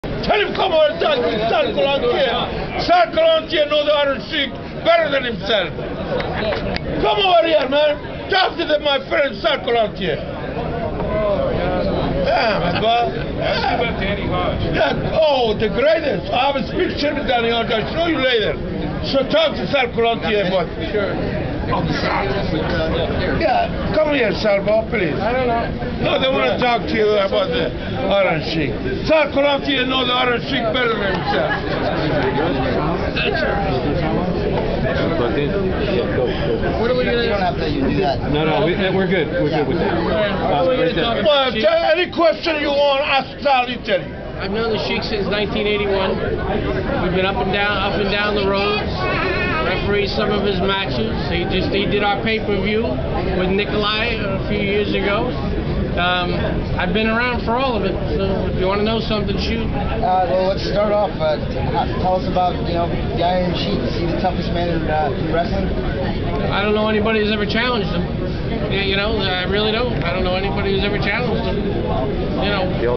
Iron Sheik with Sal
Wrestlereunion Promoter Sal has a talk with Iron Sheik